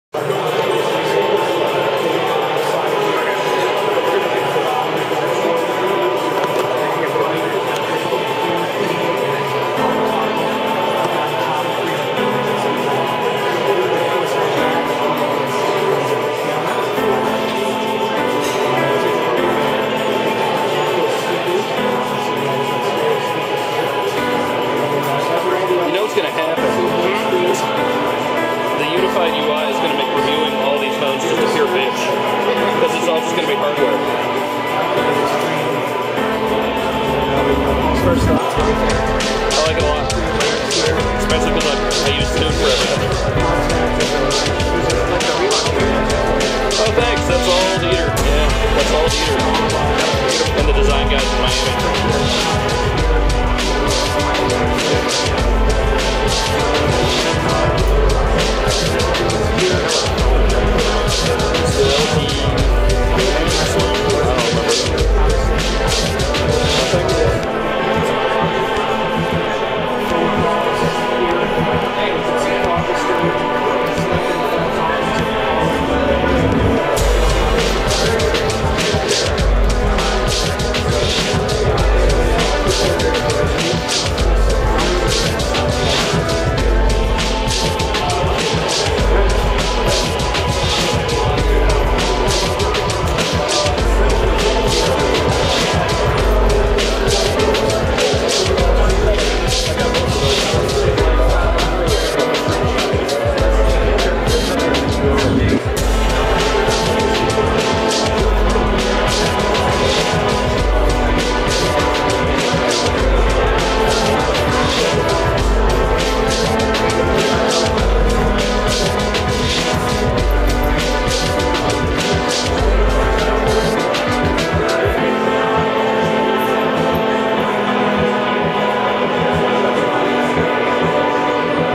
LG Quantum Windows Phone
Phil take a quick look at the LG Quantum, a Windows Phone 7 device.